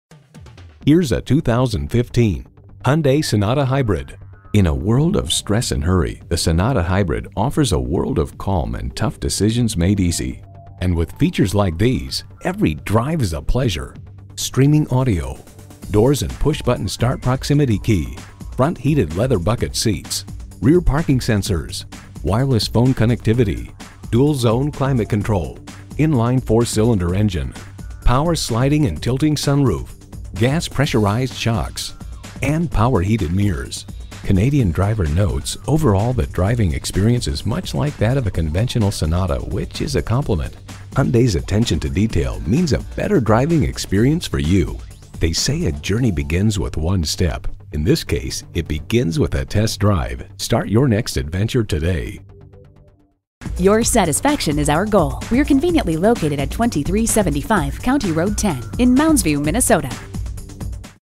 2015 Hyundai Sonata Hybrid Minneapolis MN St-Paul, MN M94130M - SOLD
Year: 2015
Make: Hyundai
Model: Sonata Hybrid
Trim: 4dr Sdn Limited
Engine: 146 L 4 Cylinder Engine Cyl.
Transmission: 6-Speed A/T
Color: Pewter Gray Metallic
Interior: Gray
Mileage: 45792
Stock #: M94130M
VIN: KMHEC4A45FA125368

Address:
2375 County Road 10